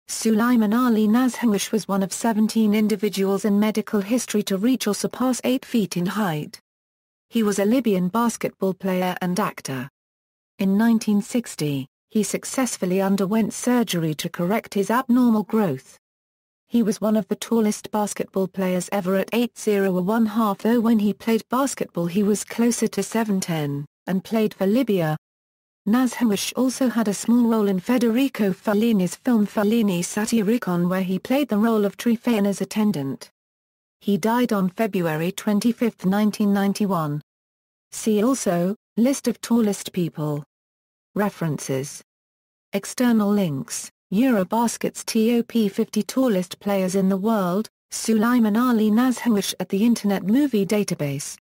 Suleiman Ali Nashnush
Suleiman Ali Nashnush was one of seventeen individuals in medical history to reach or surpass eight feet in height. He was a Libyan basketball player and actor. In 1960, he successfully underwent surgery to correct his abnormal growth. He was one of the tallest basketball players ever at 8'0½" though when he played basketball he was closer to 7'10" and played for Libya.
Nashnush also had a small role in Federico Fellini's film Fellini Satyricon where he played the role of Tryphaena's attendant. He died on February 25, 1991.